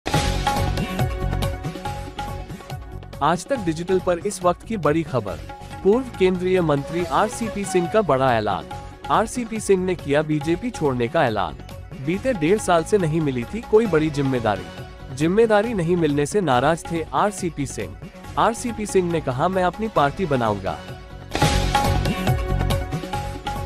Breaking News: BJP के पूर्व केंद्रीय मंत्री RCP सिंह ने बीजेपी छोड़ने का ऐलान किया | Aaj Tak News
पूर्व केंद्रीय मंत्री आरसीपी सिंह ने शनिवार को भारतीय जनता पार्टी छोड़ने का ऐलान कर दिया. उन्होंने कहा कि वो जल्द ही अपनी खुद की पार्टी बनाने की घोषणा करेंगे. आरसीपी सिंह मई 2023 में बीजेपी में शामिल हुए थे, जब बिहार के मुख्यमंत्री नीतीश कुमार ने एनडीए छोड़ दिया था और अगस्त 2022 में महागठबंधन में शामिल हो गए थे.